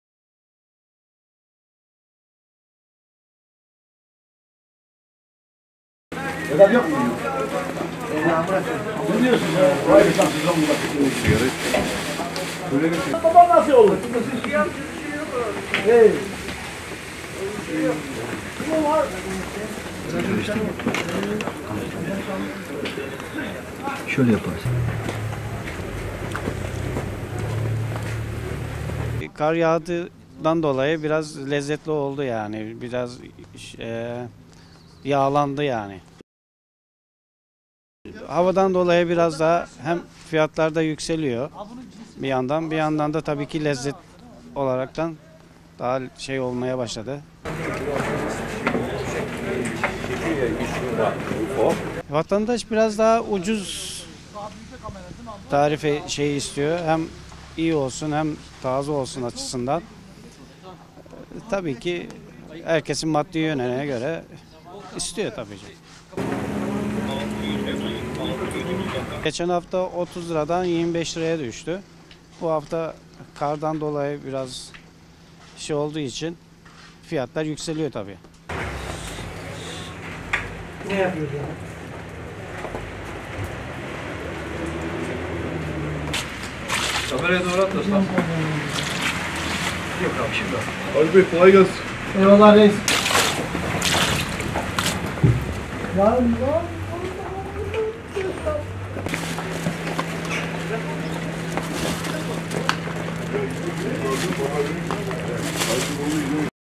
BATI KARADENİZ'DE KARLA LEZZETLENEN HAMSİ TEZGAHLARIN GÖZDESİ OLDU
Düzce'nin Akçakoca ilçesinde avlanan Karadeniz hamsisi, olumsuz hava koşulları nedeniyle fiyatı artsa da lezzetiyle en çok tercih edilen balık oldu.